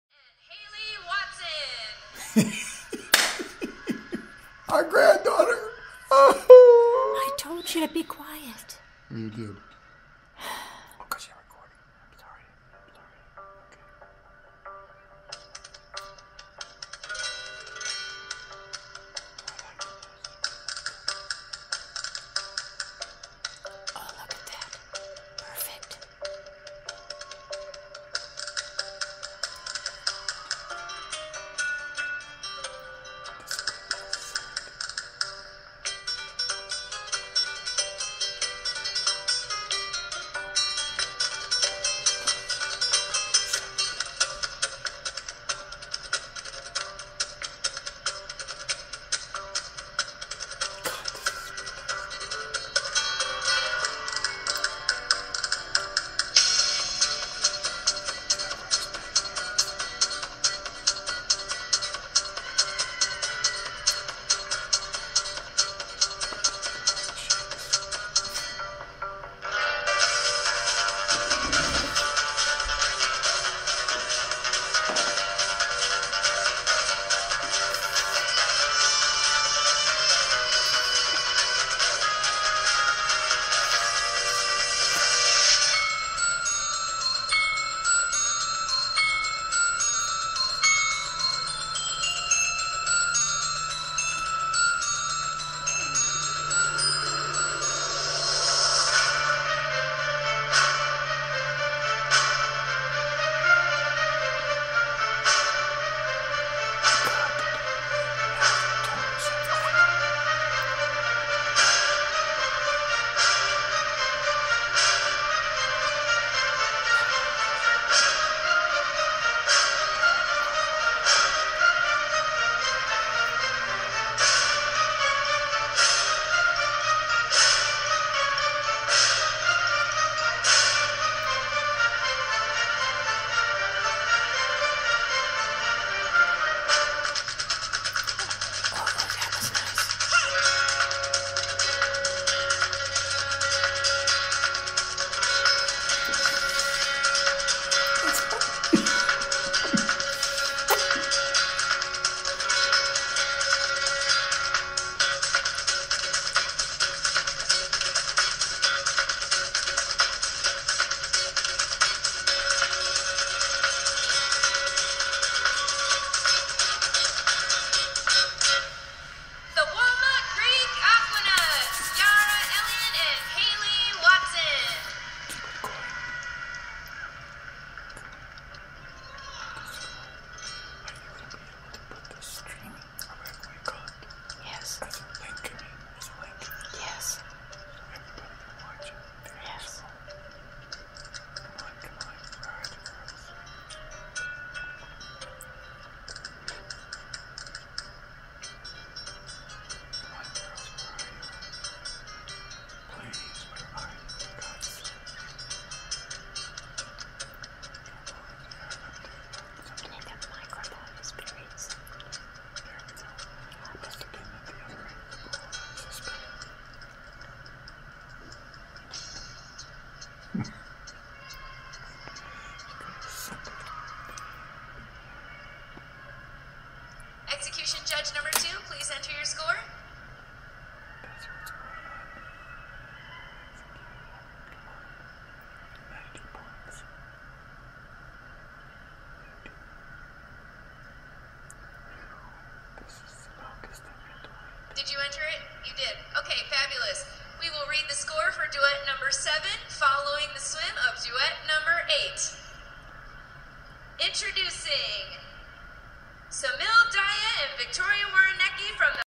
Yara Elian and Haley Watson, 13-15 Duet Bronze performance in Greensboro at the 2015 Junior Olympics Synchronized Swimming